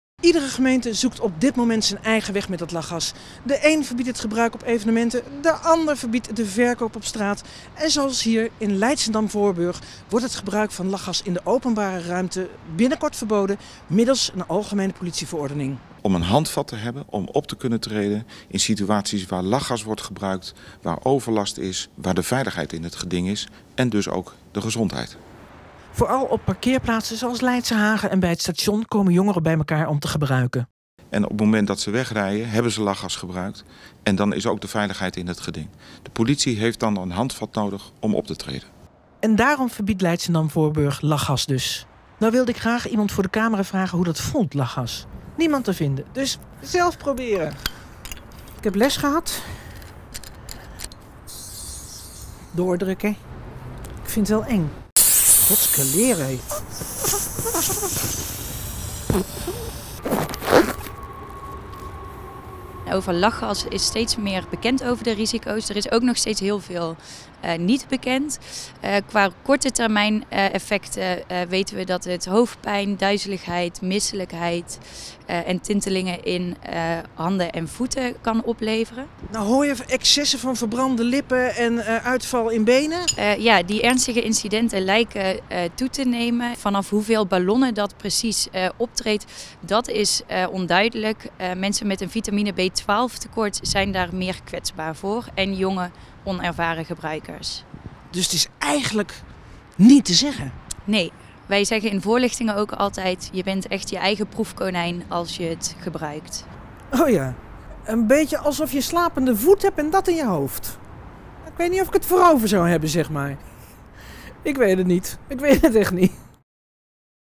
Gevaren van lachgas nog niet duidelijk - OMROEP WEST
'Er is veel bekend over lachgas, maar er is ook veel niet bekend.' Met deze woorden geeft Maud Bartelds van de Haagse instelling Indigo precies het dilemma aan rond lachgas. Indigo geeft onder andere voorlichting over verslaving. De laatste tijd is er veel te doen om lachgas. Vooral door de enorme toename in gebruik onder jongeren. 'Zolang we niet meer weten over het middel ben je je eigen proefkonijn', zijn de nuchtere woorden van Bartelds.